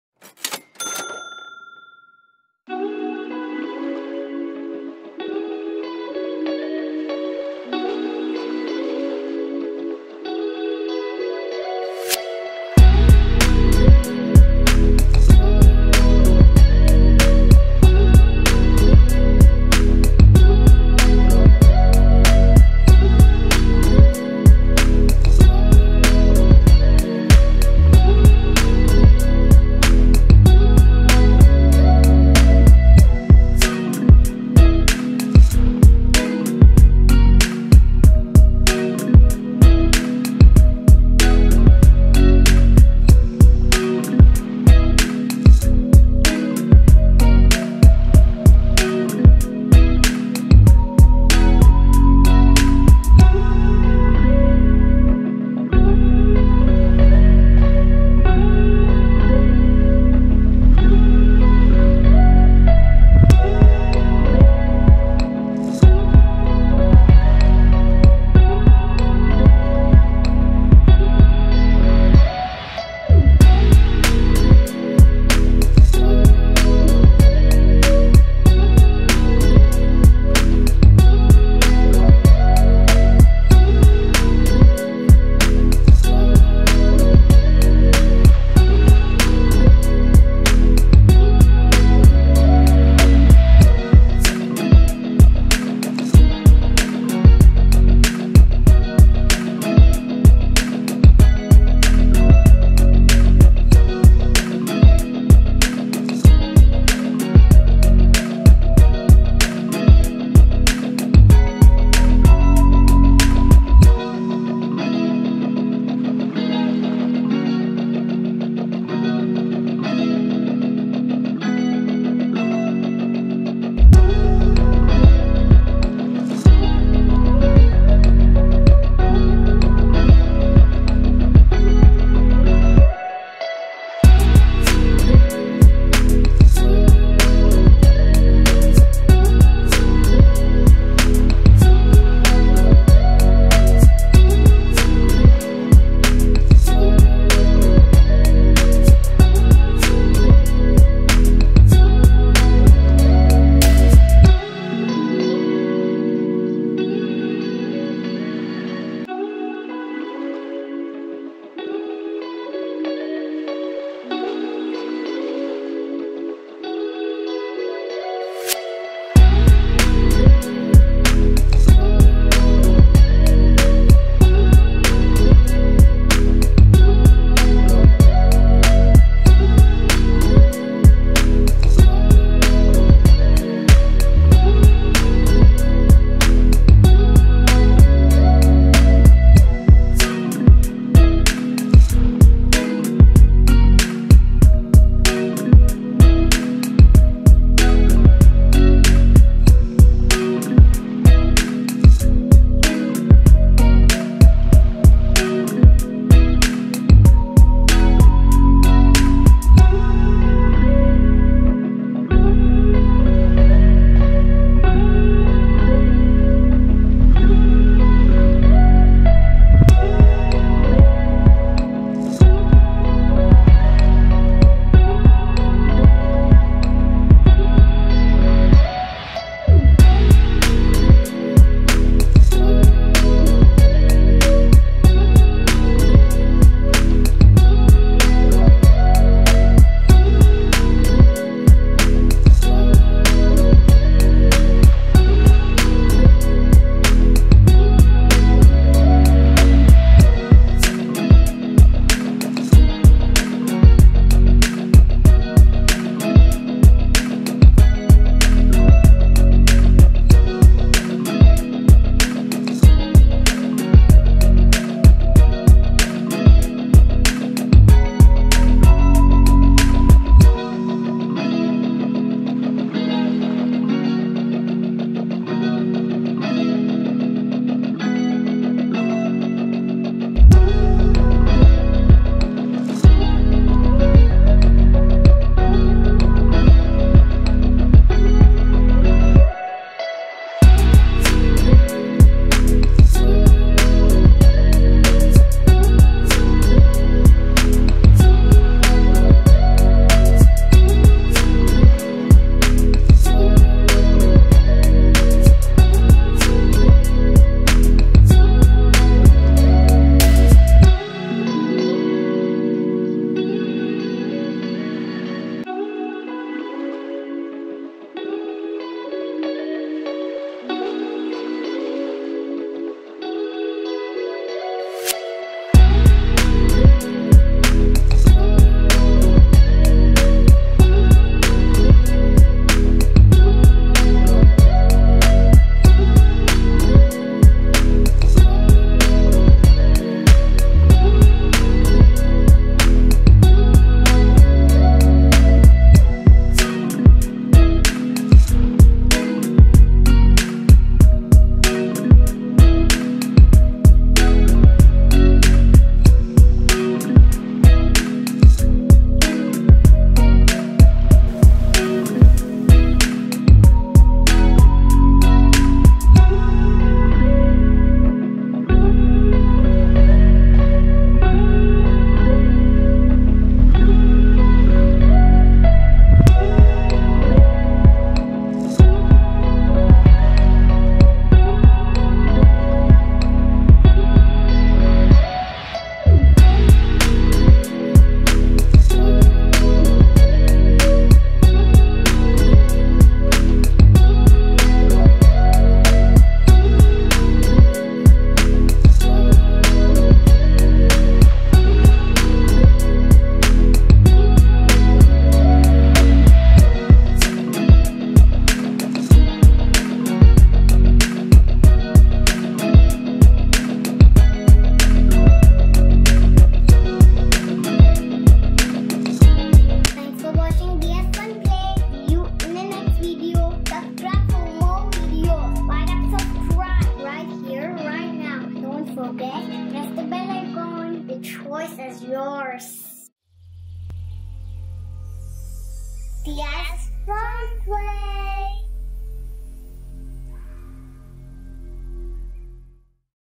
city model for science exhibition | working model | city model out of paper
Hi Friends,
In this video, you will be learning how to make a city model for high school projects and science exhibition. This model can be used as a streetlight model or a city model. Step by step tutorial for making this model is given in the video.

 This video is for educational and model making purpose.  This model will be apt for school projects, science exhibitions.  Hope you will enjoy this video

 All contents here are child friendly. There are loads of information to learn and play.